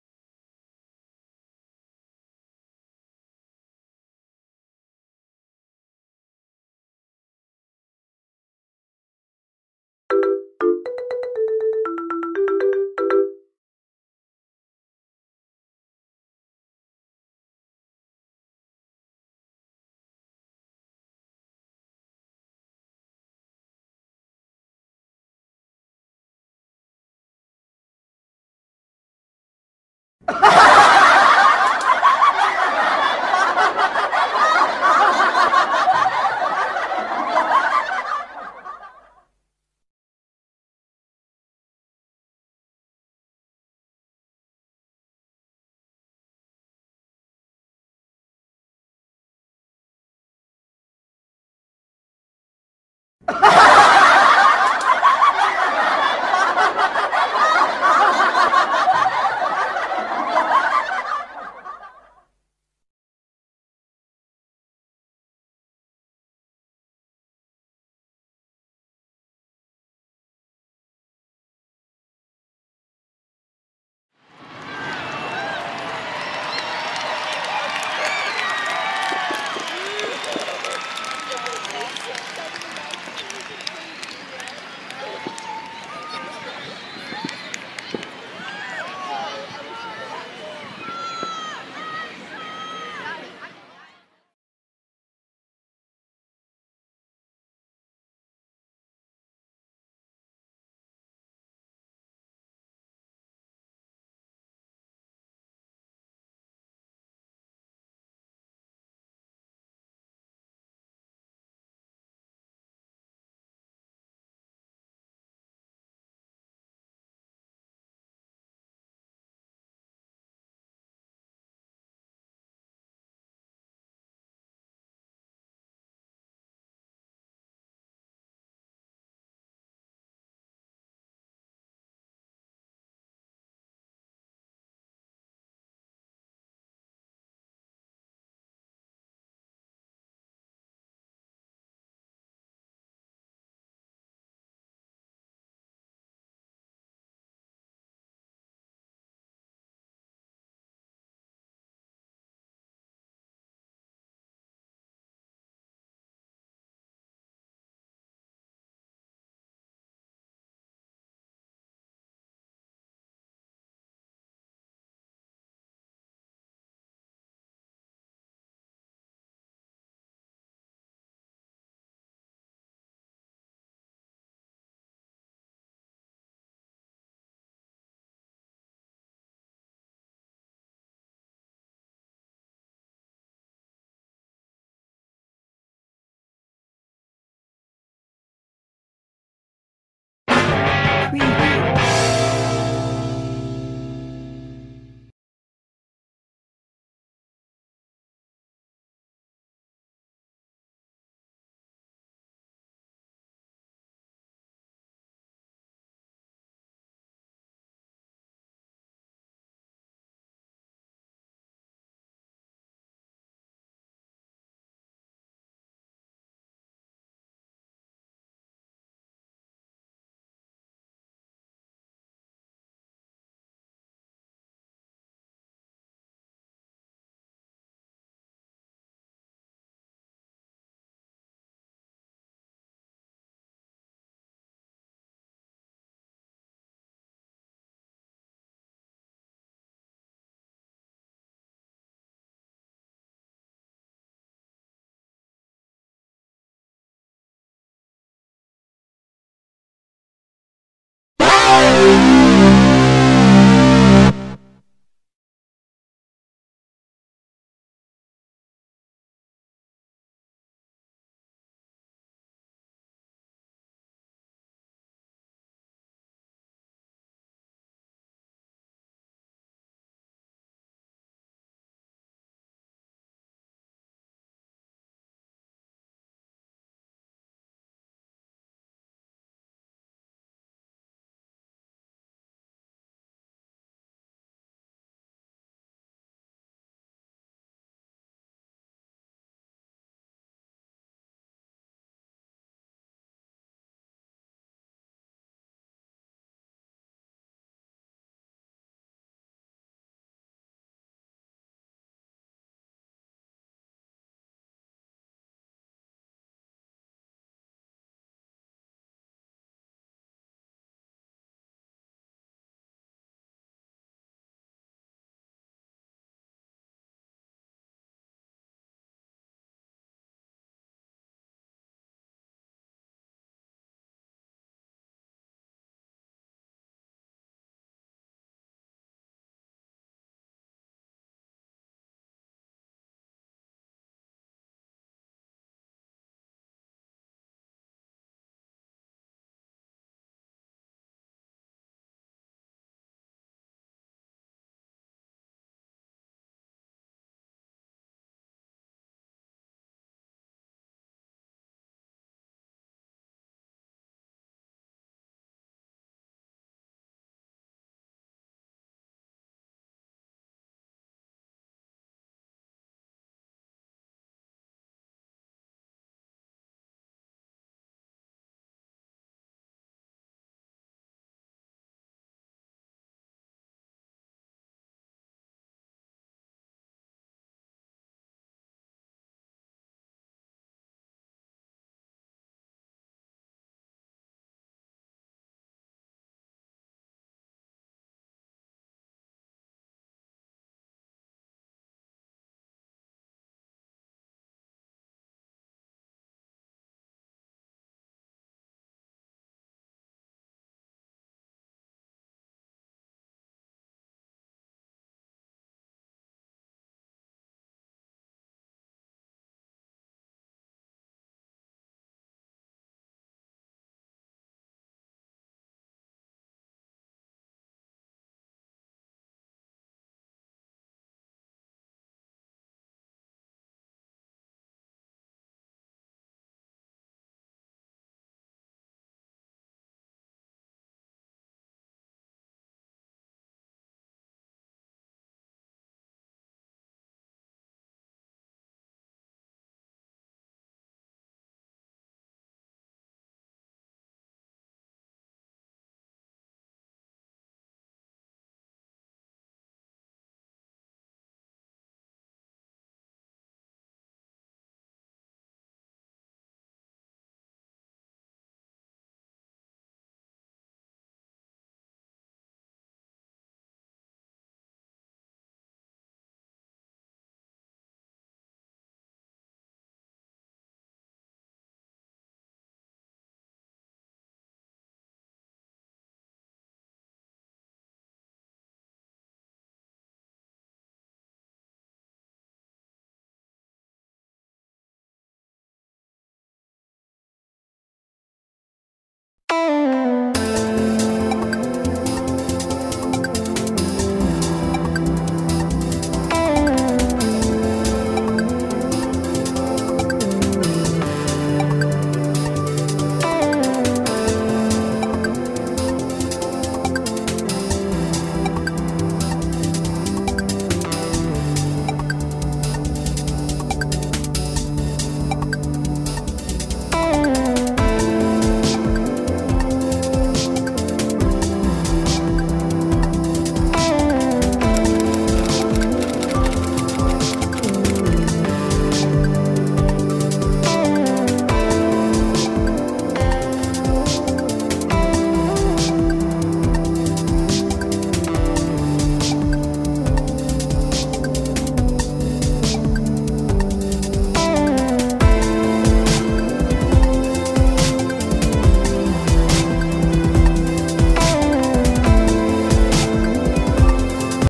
Just Rippin' on the Flash and His Box Office Turd
A bit of comedy in the form of a slideshow. Sometimes I like to dip my toes into the topics from other outlets. Sometimes I like to make jokes at the expense of a controversial celebrity. But at the end of the day, we're just talking about a very bad actor starring in a very bad movie. I don't like to advertise products that I don't use at all.